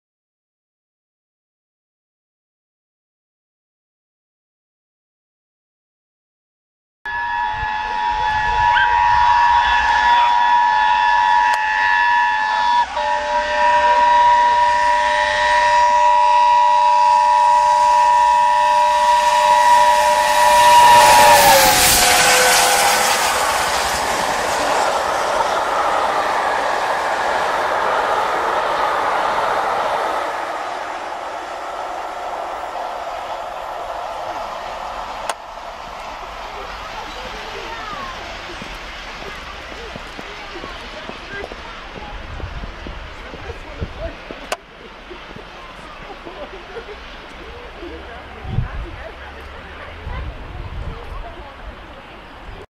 Devon Belle Tornado 60163 passing Castle Cary 25 Aug 2014
LNER Peppercorn Class steam locomotive no 60163 'Tornado' racing noisily through Castle Cary Station on the return UK Railtours excursion 'Devon Belle' from Exeter to Paddington on 25th August 2014.